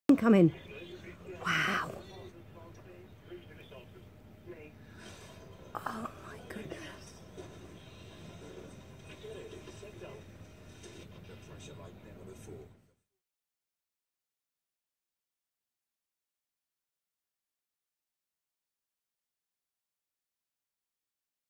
Squirrel bursts out of a box of nuts like a 'jack in the box'
The cheeky critter is captured trying to keep a whole feeder of peanuts to itself, hiding inside it and jumping out when other squirrels are close by. It was filmed at Lakeland Caravan Park in Norfolk. Retired flower shop owner, Julie Malleson, 58, captured the clip during a week-long break with her husband, Adrian.